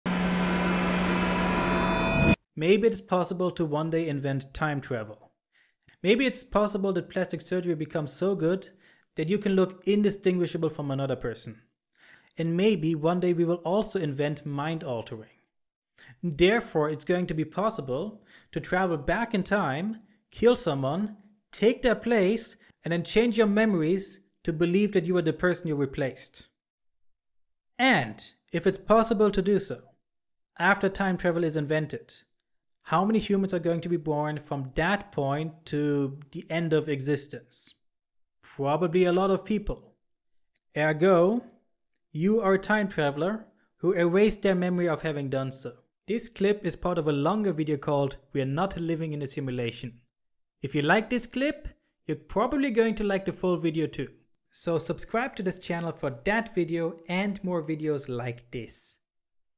Proof that you are a time traveler who erased their memories of having done so
This is part of a video that examines the simulation theory by Nick Bostrom, and why it falls short. No, we are not living in a simulation.